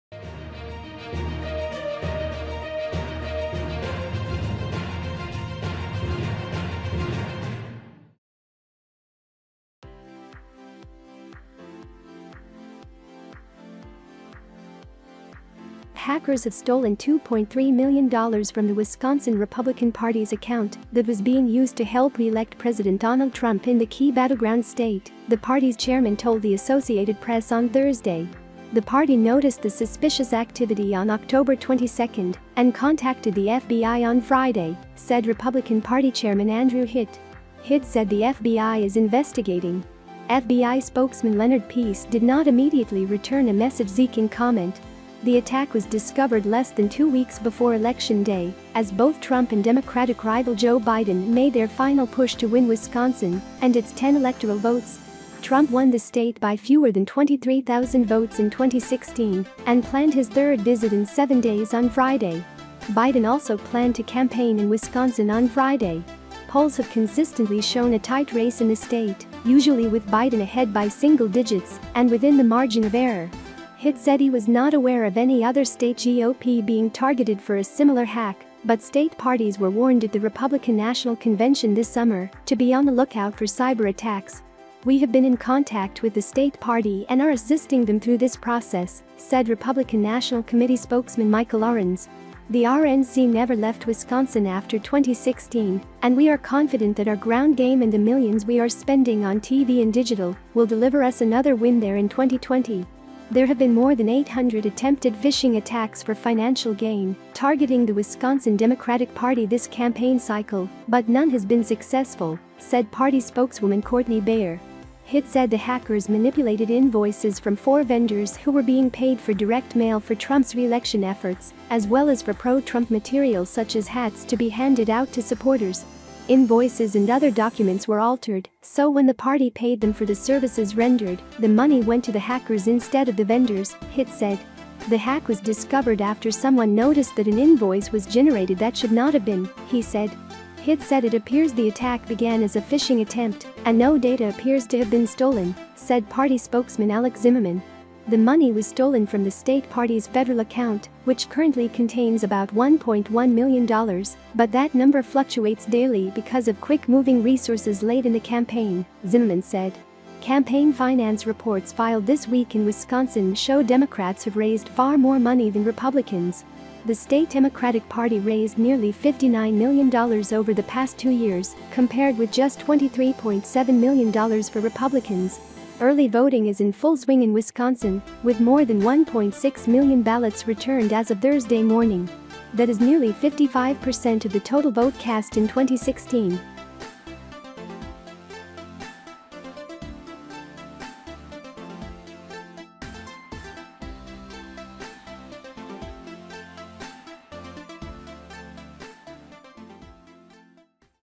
Wisconsin Republican Party says hackers stole $2 3 million | World News Today
Hackers have stolen $2.3 million from the Wisconsin Republican Party's account that was being used to help reelect President Donald Trump in the key battleground state, the party's chairman told The Associated Press on Thursday.

The party noticed the suspicious activity on Oct. 22 and contacted the FBI on Friday, said Republican Party Chairman Andrew Hitt.

Hitt said the FBI is investigating. FBI spokesman Leonard Peace did not immediately return a message seeking comment.

The attack was discovered less than two weeks before Election Day, as both Trump and Democratic rival Joe Biden made their final push to win Wisconsin and its 10 electoral votes. Trump won the state by fewer than 23,000 votes in 2016 and planned his third visit in seven days on Friday. Biden also planned to campaign in Wisconsin on Friday. Polls have consistently shown a tight race in the state, usually with Biden ahead by single digits and within the margin of error.

Hitt said he was not aware of any other state GOP being targeted for a similar hack, but state parties were warned at the Republican National Convention this summer to be on the lookout for cyber attacks.